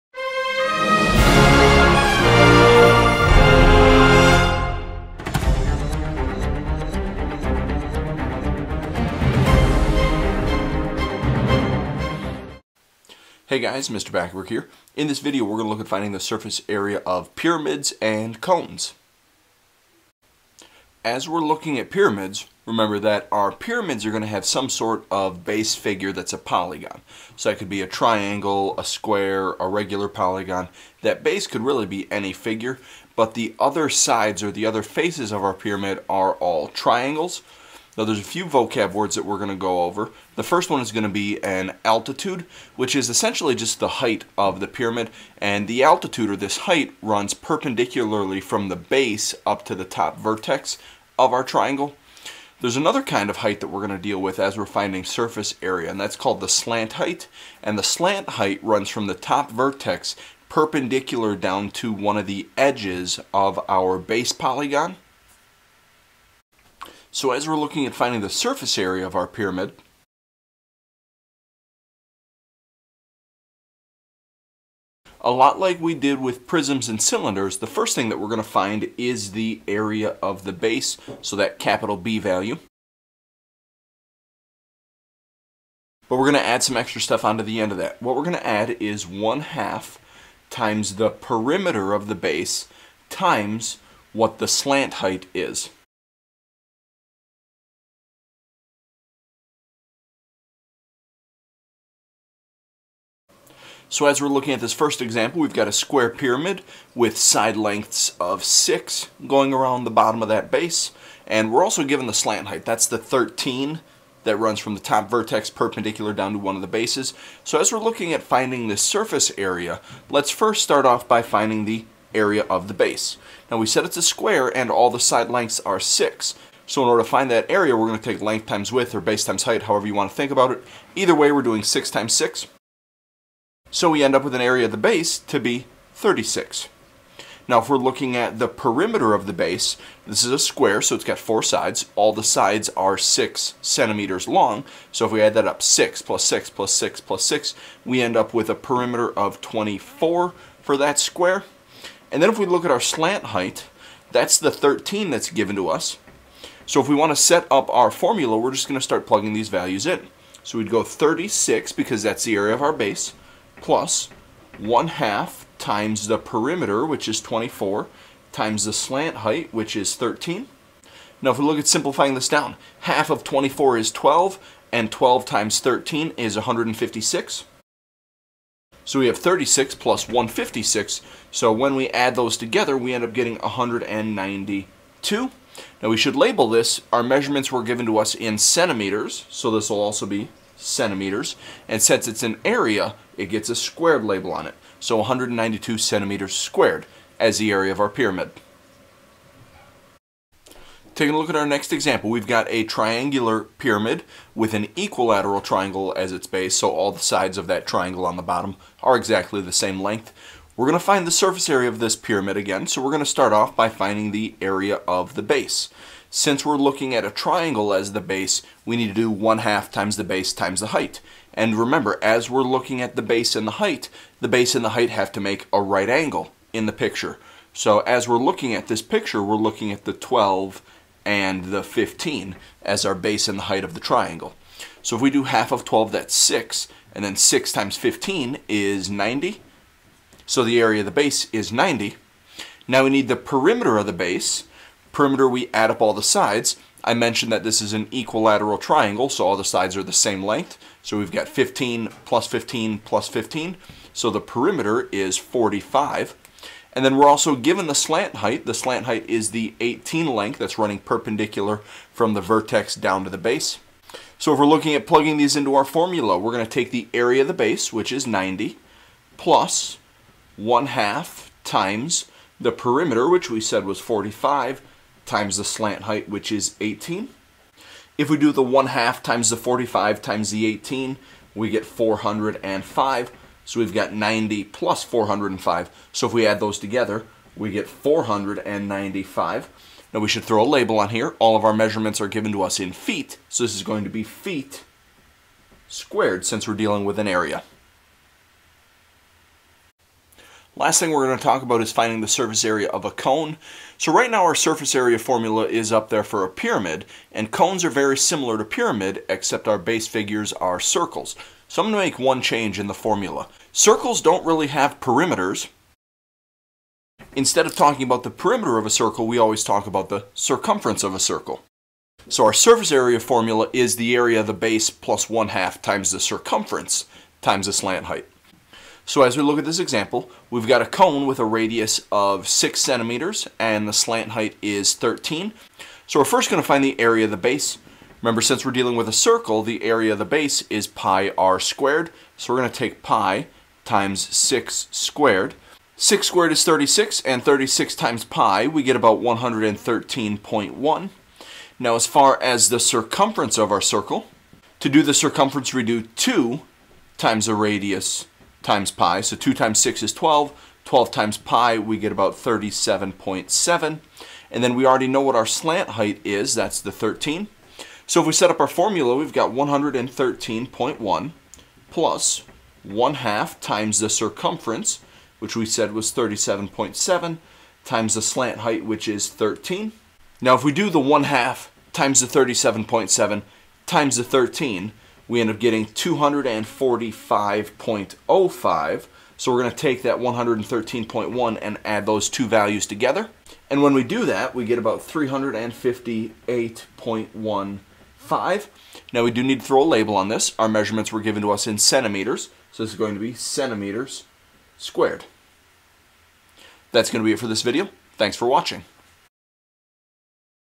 Geometry 11.7: Pyramids and Cones part 1
Objective:

Find the surface area of pyramids and cones.